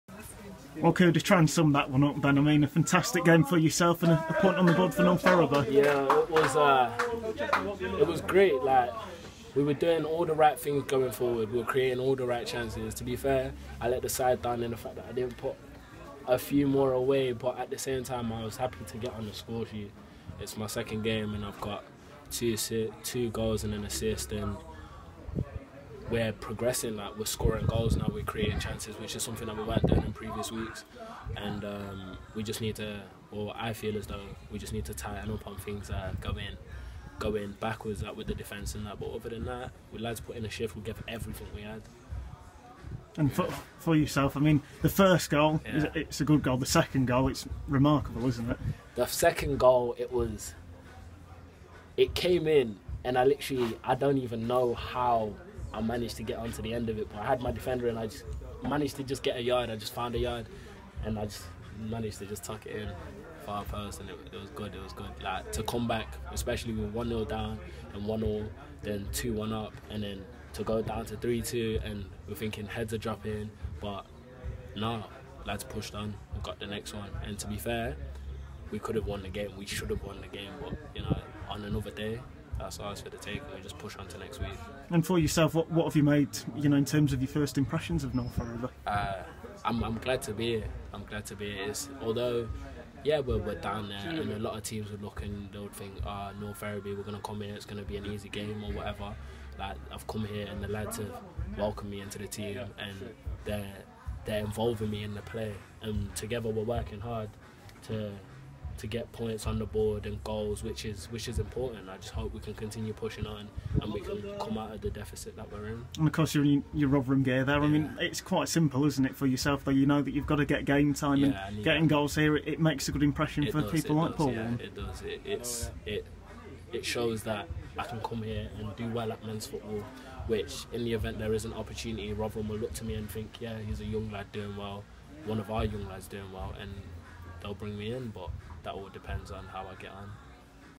Kuda Muskwe on his time at North Ferriby United so far
The North Ferriby United loanee spoke to Tom Feaheny.